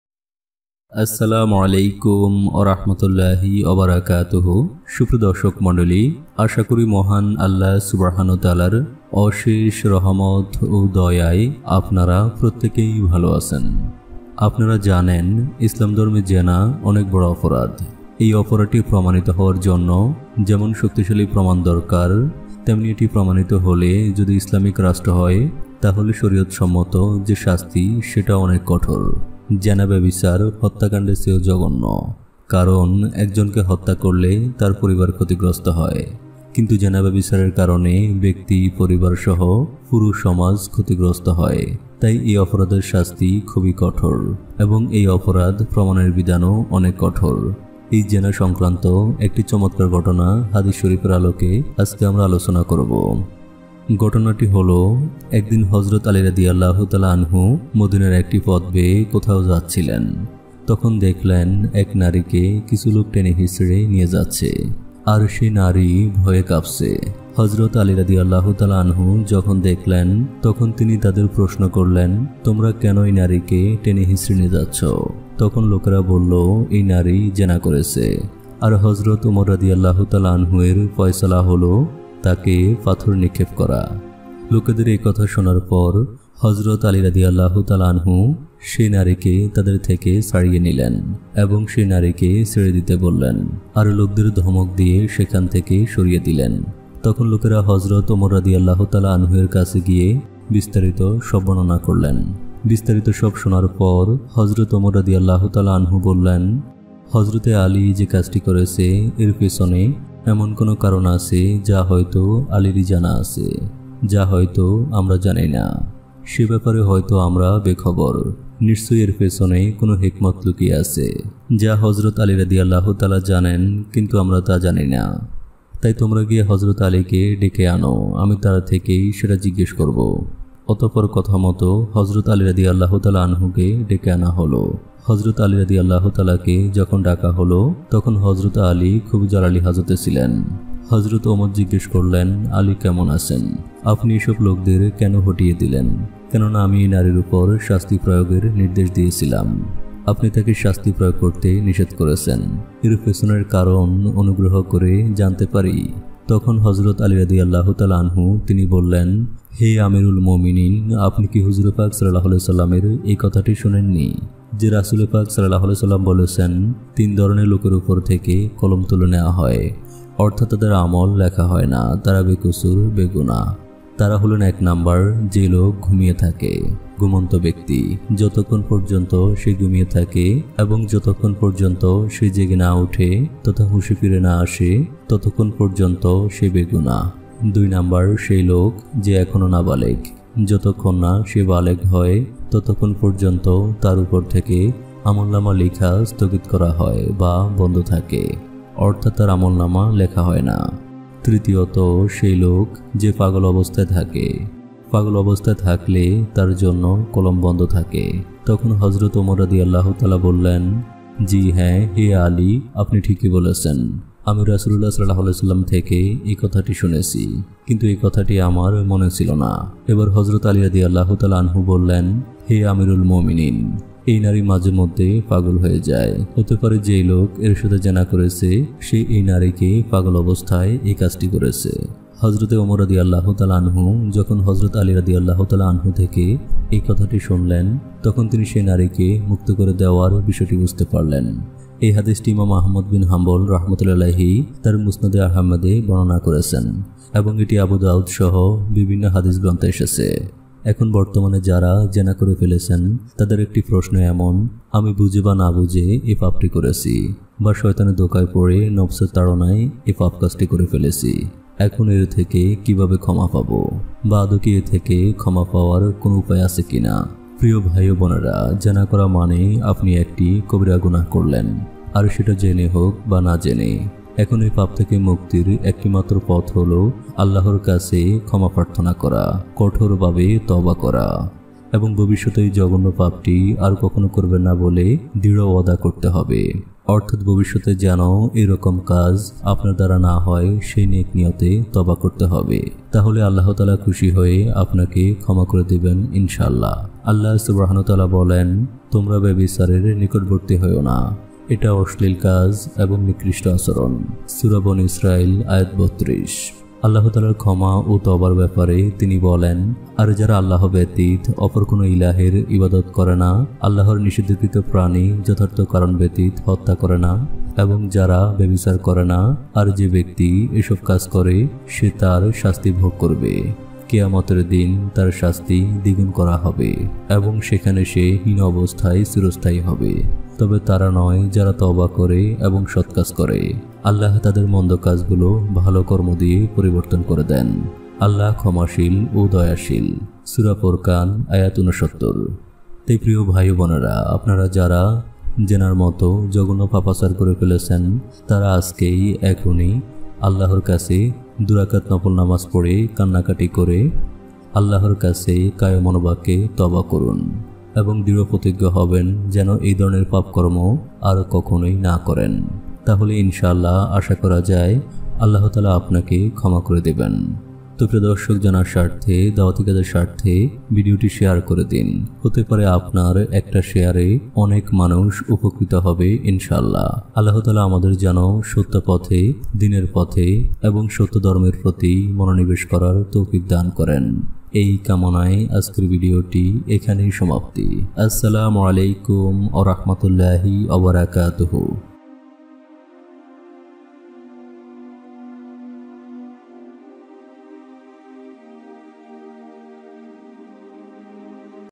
৩ প্রকারের লোক যেনা করেও নির্দোষ বা নিষ্পাপ! মা বোন আমাদের সকলের জানা জরুরি
আপনারা জানেন ইসলাম ধর্মে জেনা অনেক বড় অপরাধ। এই অপরাধটি প্রমাণিত হওয়ার জন্য যেমন শক্তিশালী প্রমাণ দরকার তেমনি এটি প্রমাণিত হলে যদি ইসলামিক রাষ্ট্র হয়, তাহলে শরীয়ত সম্মত যে শাস্তি সেটাও অনেক কঠোর।

প্রিয় ভাই ও বোনেরা আপনারা যারা যেনার মতো জগন্য পাপাচারটি করে ফেলেছেন; তাঁরা আজকেই এখুনি আল্লাহ্‌র কাছে দু রাকাআত নফল নামাজ পড়ে কান্নাকাটি করে আল্লাহ্‌র কাছে কায়মনোবাক্যে তওবা করুণ। এবং দৃঢ় প্রতিজ্ঞ হবেন যেন আপনারা এই পাপ কাজটি আর না করেন। তাহলে ইনশাআল্লাহ্‌ আশা করা যায় আল্লাহ্‌ তায়ালা আপনাকে ক্ষমা করে দিবেন।